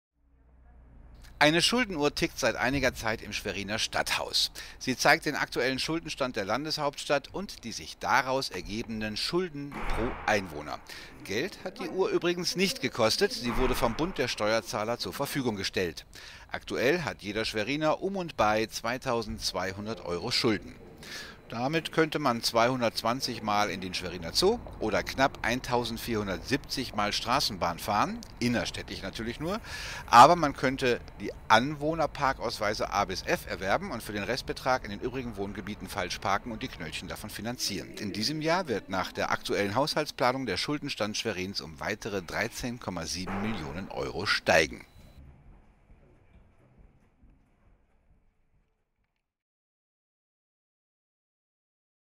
Schulden Uhr
Eine Schuldenuhr tickt seit einiger Zeit im Schweriner Stadthaus. Sie zeigt den aktuellen Schuldenstand der Landshauptstadt und die sich daraus ergebenden Schulden pro Einwohner an. Geld hat die Uhr übrigens selbst nicht gekostet. Sie wurde vom Bund der Steuerzahler zur Verfügung gestellt. Aktuell hat jeder Schweriner 2.200,. Euro Schulden. Damit könnte man 220 mal in den Schweriner Zoo oder knapp 1470 mal Straßenbahn fahren, innerstädtisch natürlich nur, man könnte die Anwohnerparkausweise A bis F erwerbenund für den Restbetrag in den übrigen Wohngebieten falsch parken und die Knöllchen davon finanzieren. Übrigens: in diesem Jahr wird nach der aktuellen Haushaltsplanung der Schuldenstand Schwerins um weitere 13,7 Millionen steigen.